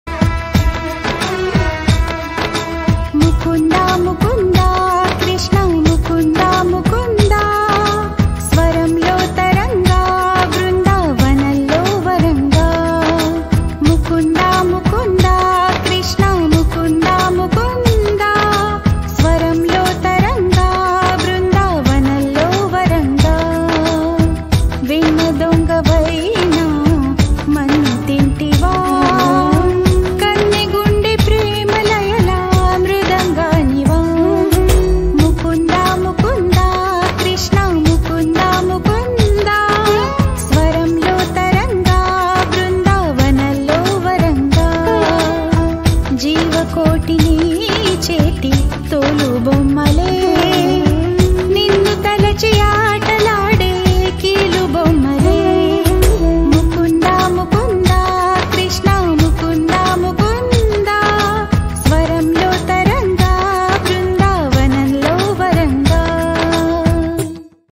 Mayookha as Little Shri Krishna | చిన్ని కృష్ణయ్యగా నా కూతురు మయూఖ
My Daughter Mayookha dressed as Little Shri Krishna.